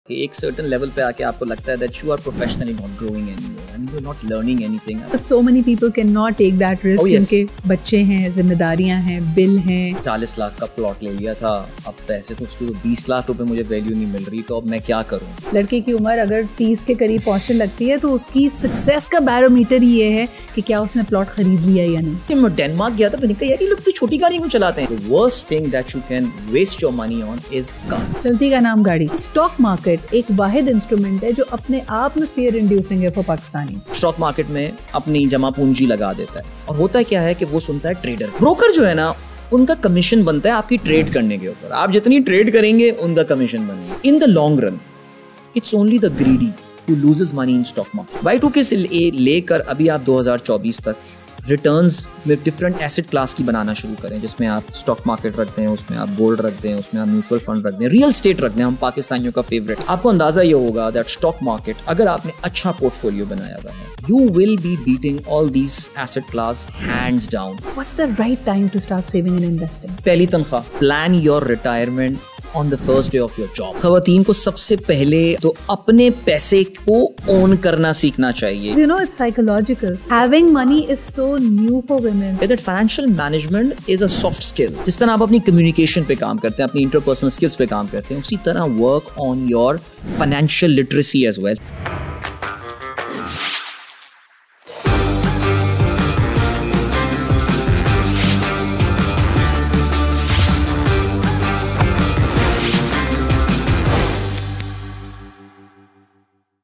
How to make Savvy Investments for a Secure Future? | Adeel Ahmed | Sidra Iqbal | Promo
In this promo, Adeel Ahmed shares valuable insights on how to make smart investments that ensure a secure future. Joined by Sidra Iqbal, they discuss strategies, financial tips, and the key to making wise decisions in the ever-changing investment landscape.